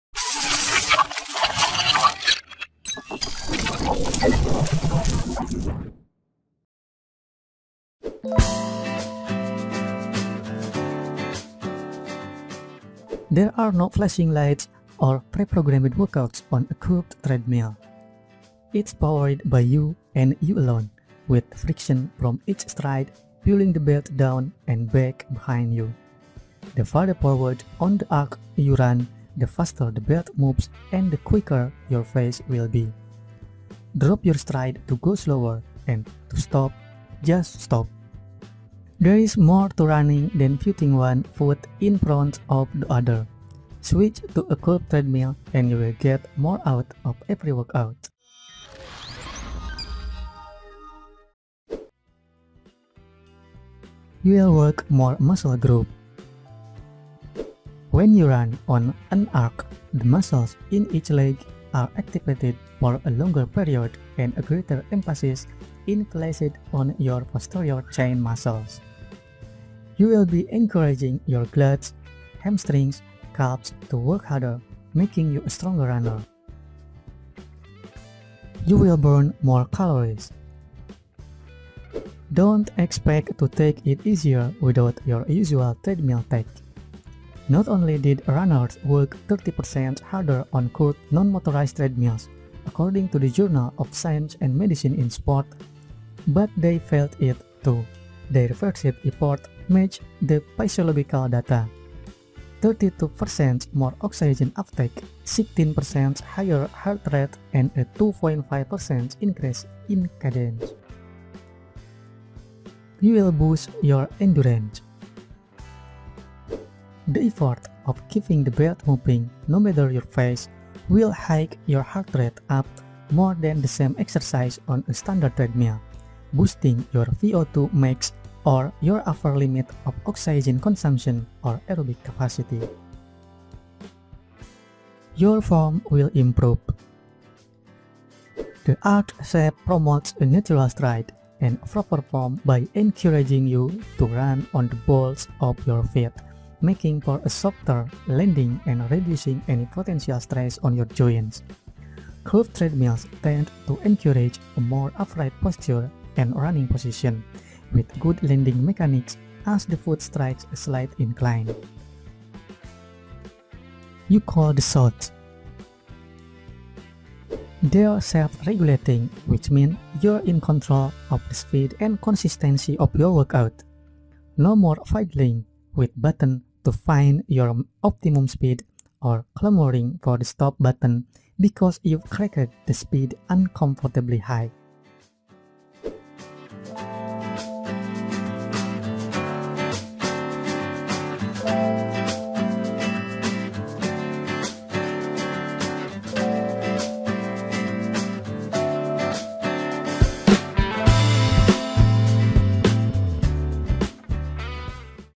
What Are the Benefits of a Curved Treadmill?
There are no flashing lights or pre-programmed workouts on a curved treadmill. It’s powered by you and you alone, with the friction from each stride pulling the belt down and back behind you. The further forward on the arc you run, the faster the belt moves and the quicker your pace will be. Drop your stride to go slower, and to stop, just… stop.

There’s more to running than putting one foot in front of the other. Switch to a curved treadmill and you’ll get more out of every workout. Here’s how...